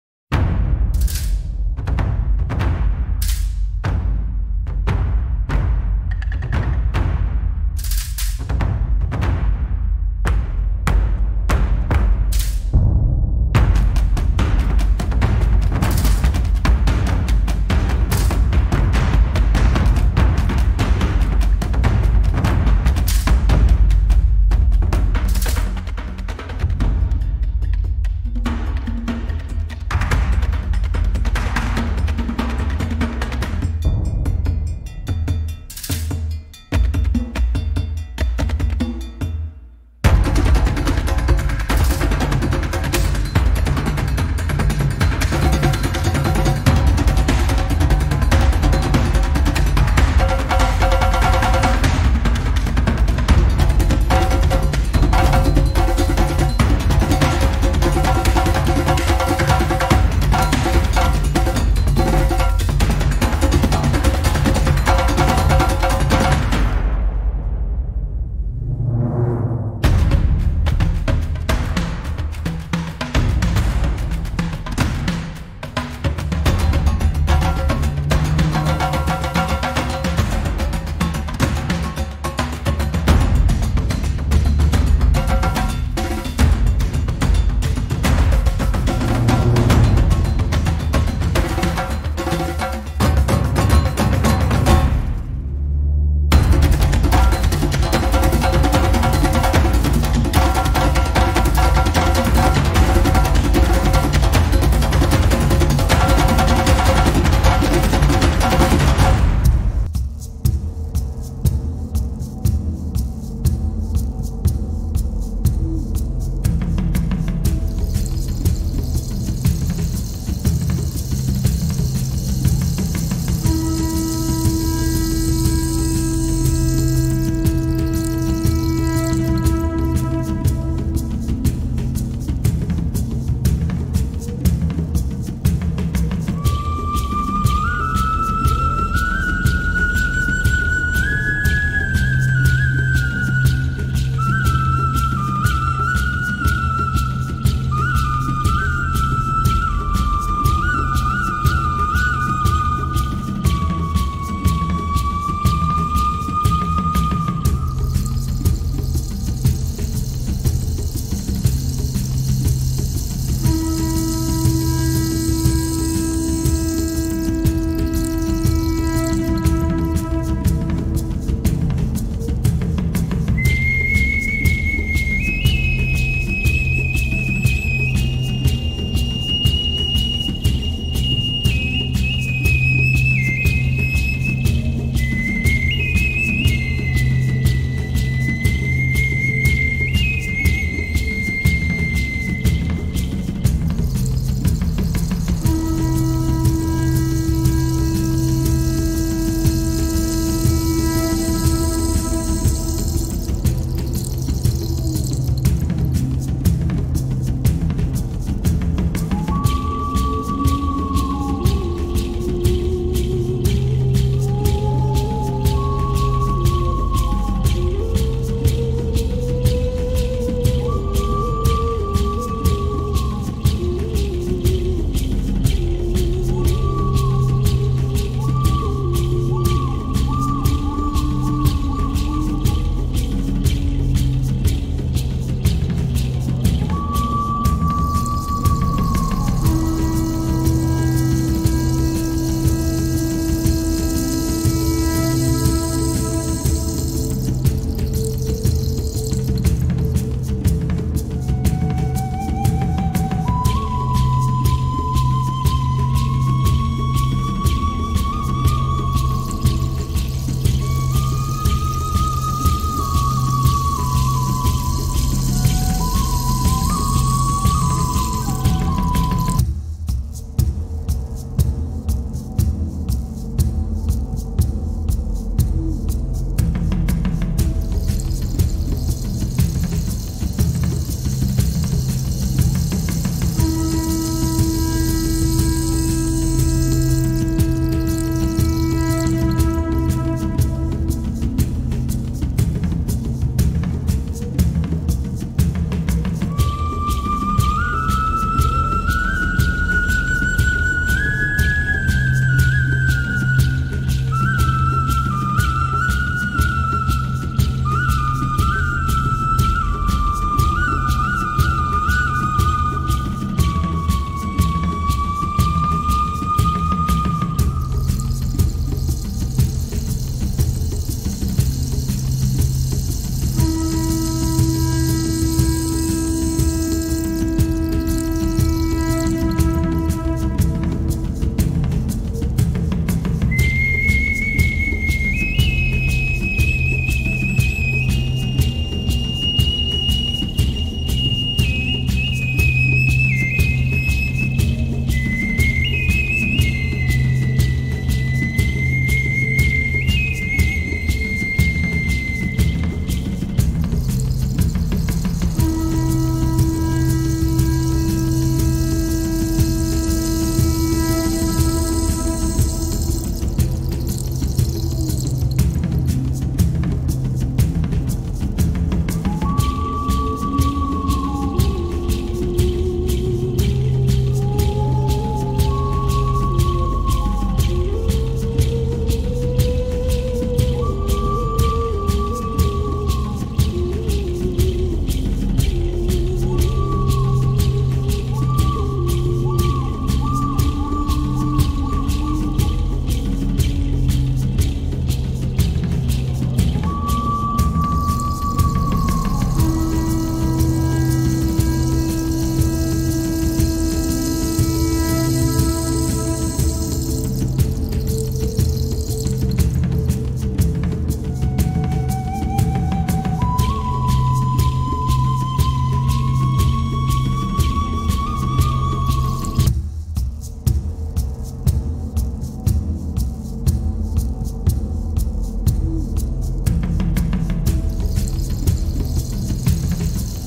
Amazing wild life!
The lion, known as the "king of the jungle," is a powerful predator with a majestic mane and a commanding presence. Tigers, the largest of big cats, are solitary hunters with bold, orange coats and black stripes. Elephants, the world's largest land animals, are intelligent and social, recognized for their long trunks, tusks, and massive size. Giraffes, the tallest land animals, have long necks and legs, allowing them to reach high tree canopies for food. Each of these animals plays a vital role in their ecosystems, symbolizing strength, grace, and the diversity of wildlife.